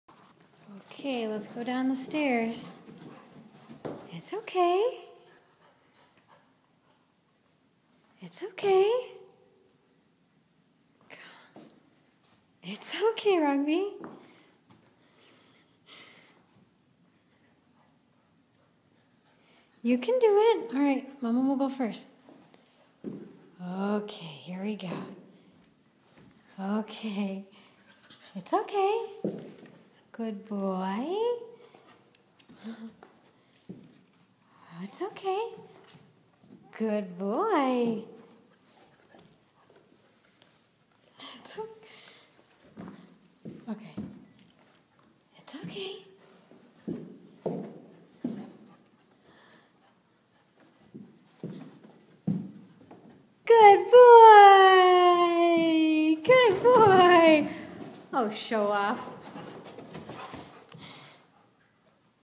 Uncle shows off
Old English Sheepdog puppy going down the stairs for the first time!